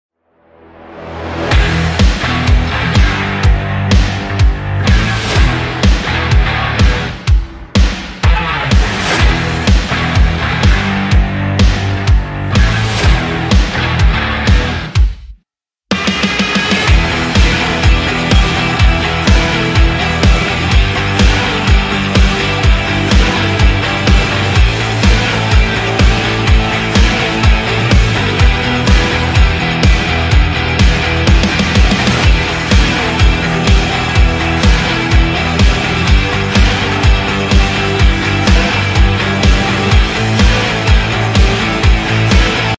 2020 American Heritage Vs Benjamin School Nick Swanson Highlight #65
American Heritage Vs Benjamin School 2020 Football Film

Nick Swanson is a 6’2” 304lbs offensive guard for American Heritage Boca Delray Campus. This is his highlight tape from his junior season. He is based in Florida